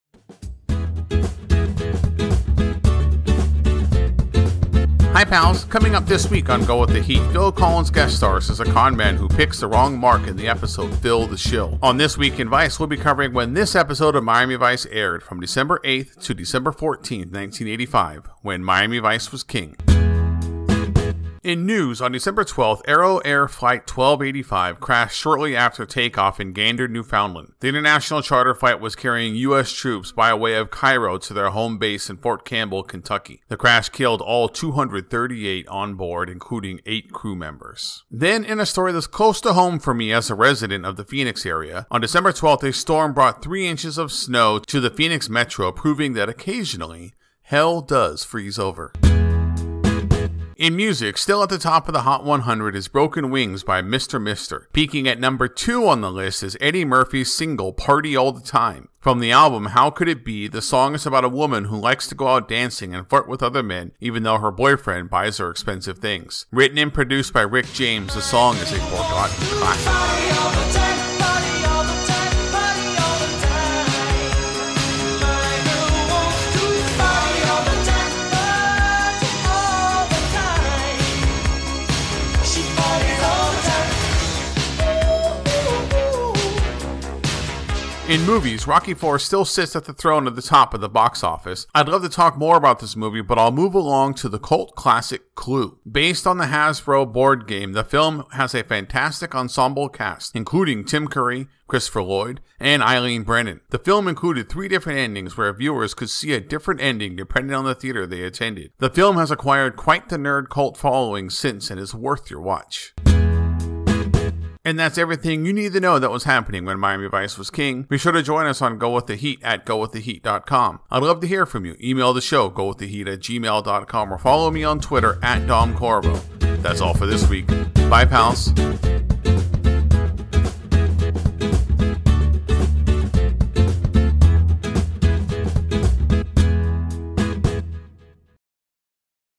This Week In Vice 22 - Dec. 8 - 14, 1985
Hi pals, we've changed our schedule a little and will now be releasing This week in Vice before the main episode as a little teaser for what's coming.

Coming up this week on Go With The Heat Phil Collins guest stars as a conman who picks the wrong mark in the episode *Phil The Shill*. On This Week in Vice we'll be covering when this episode of Miami Vice aired, from Dec. 8 to Dec. 14, 1985, when Miami Vice was King.

Movie:
Clue